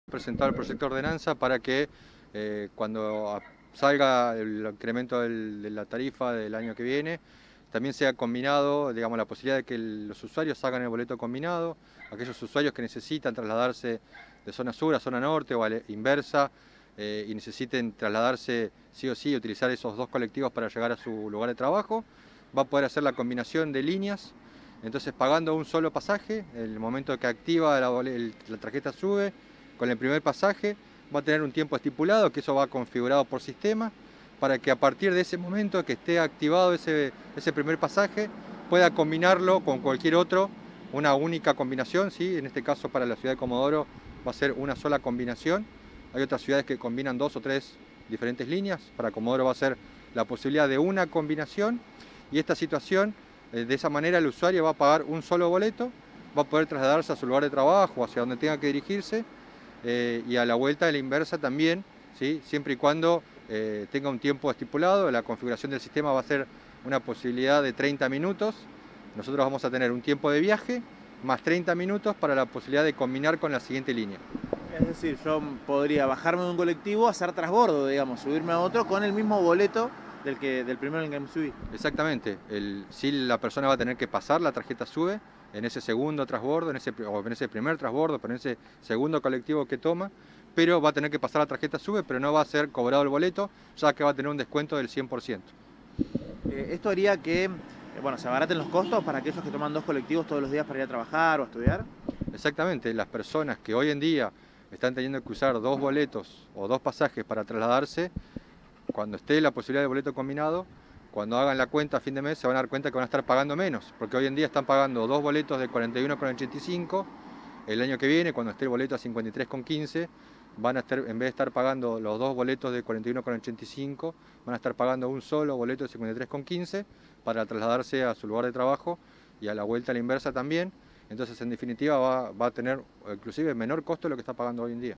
Colectivos: En Comodoro se implementaría el boleto combinado
En el proyecto de ordenanza que prevé un aumento en el transporte publico, esta contemplada la idea de implementar boletos combinados, para aquellas personas que toman dos colectivos para moverse entre zona norte y sur. La idea es que puedan hacerlo con un solo boleto en ambos tramos, y no tener que sacar uno en cada colectivo. Esto reduciría el gasto.